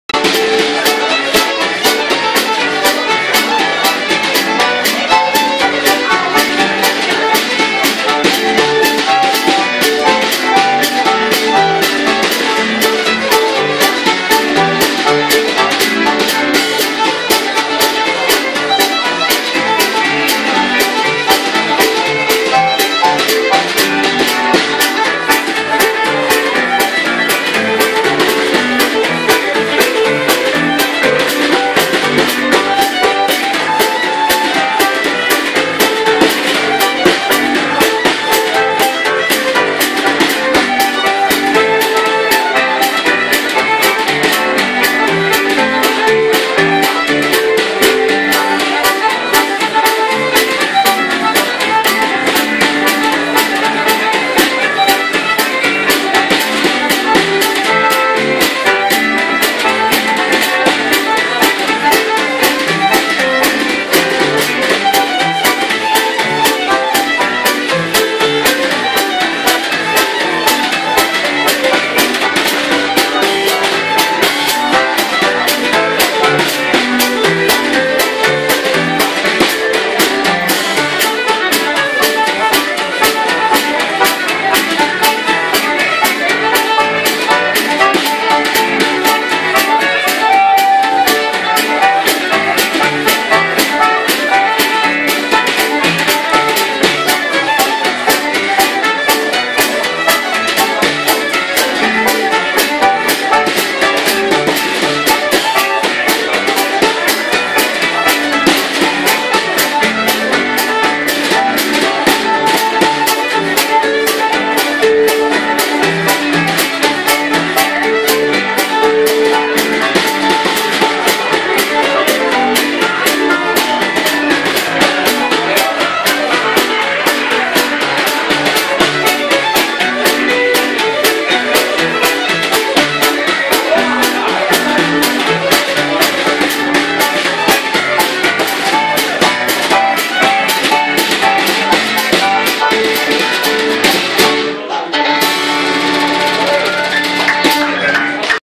Neil Dawson Fiddle Reels
Neil Dawson, Graham Crichton, Tom Orr and John Burns in the Central Bar in Arbroath. July 2013